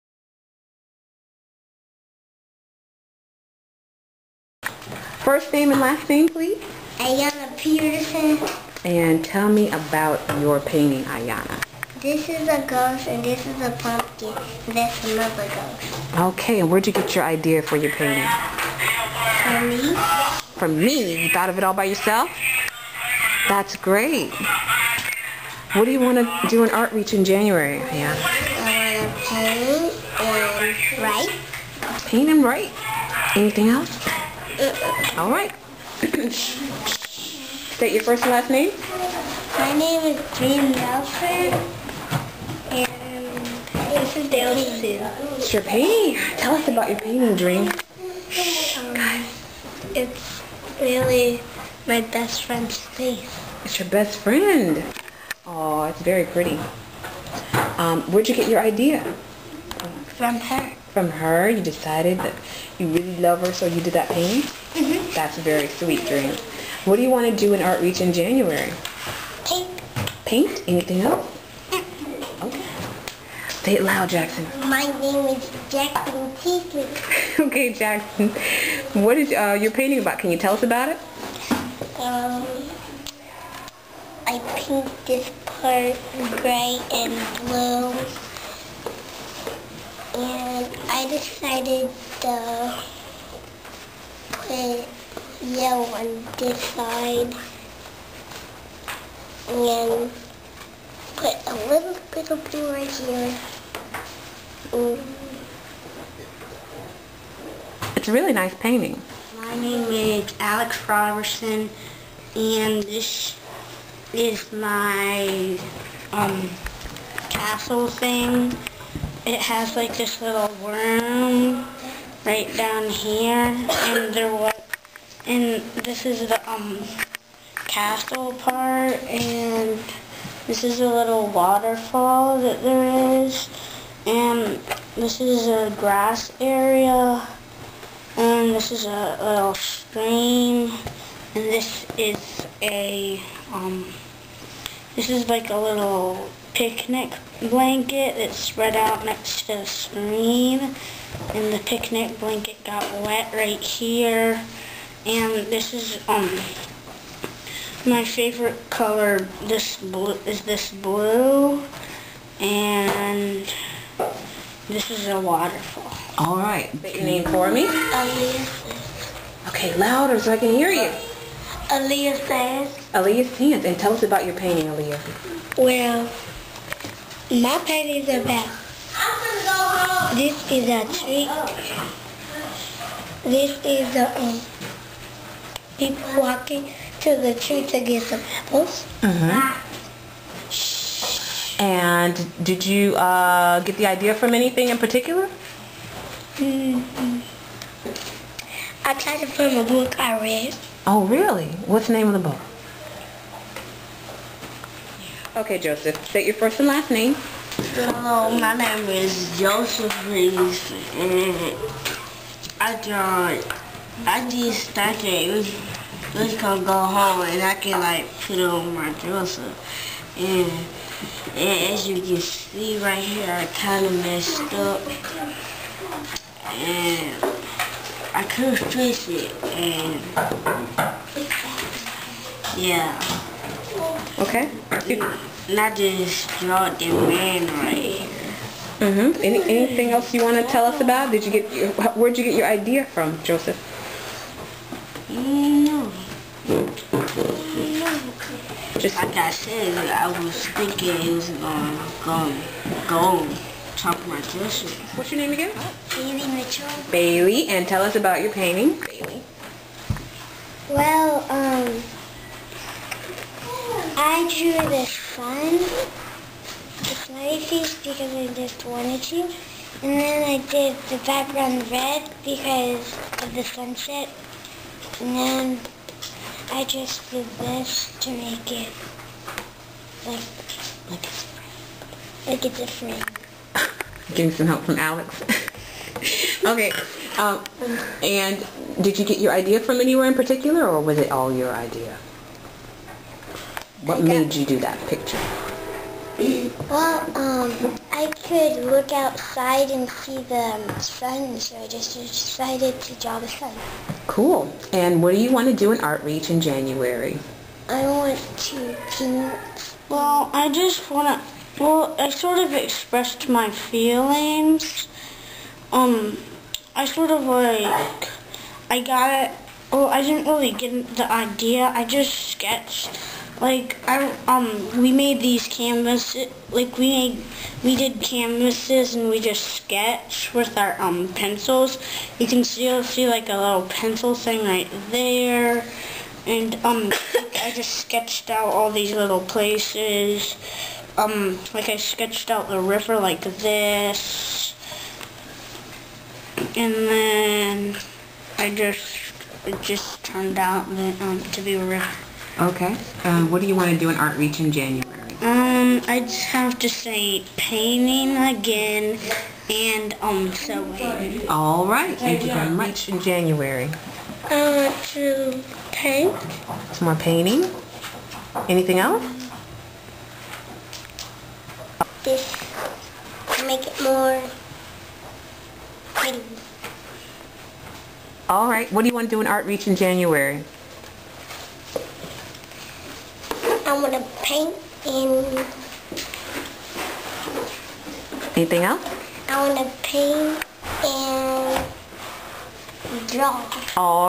ArtReach 2009.wmv
November 2009. ArtReach participants discuss their paintings from the previous week.